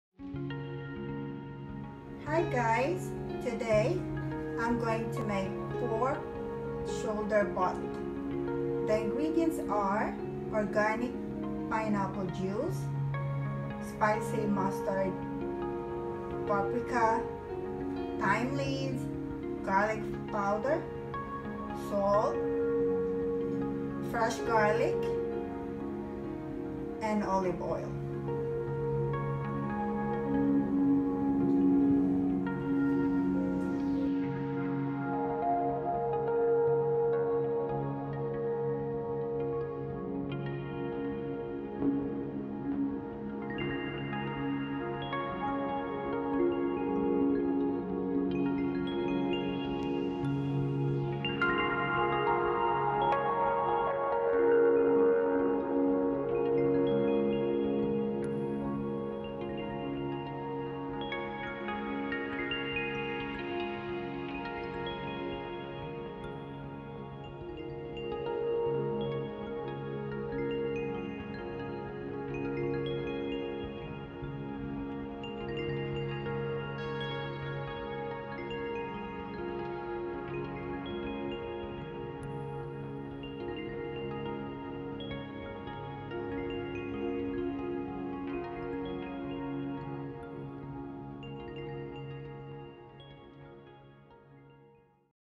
Organic Pork Shoulder Butt | Surprise Meal for a friend !
My Organic Ingredients are:

 10 lbs of Pork shoulder Butt
1 Cup Simple Supreme Spicy Brown Mustard
1 tbsp  Paprika
1 tsp   Thyme Leaves
1 tbsp  Garlic Powder
6 pcs Fresh chopped Garlic
1 tbsp Himalayan Salt
1/4 cup Olive Oil
Mix all Together then rubbed on

Add Organic  3 cups of Pineapple Juice for basting (button Pan)

#   Bake 300 degrees per pound

#   Last 5 to 10 mins= Bake 450 degrees to make it Crispy

We also give him ICE Cream Cake :) :)

Options:
You can Add 2 tbsp brown sugar  & Black pepper

I hope you Enjoy my video :)  Please  PRAY for him as well.

May God Bless you and keep you Safe.